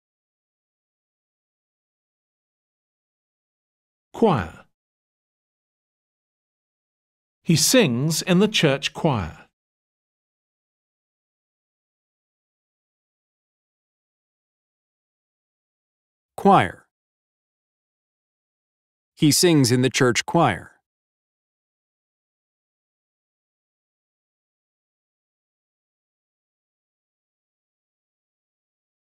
How to pronounce choir | British English and American English pronunciation
Learn how to pronounce "choir" in British English and American English. Hear the pronunciation of the word on its own and in example sentences.

Look up the meaning of “choir” and thousands of other words and pronunciations for free in the Cambridge Dictionary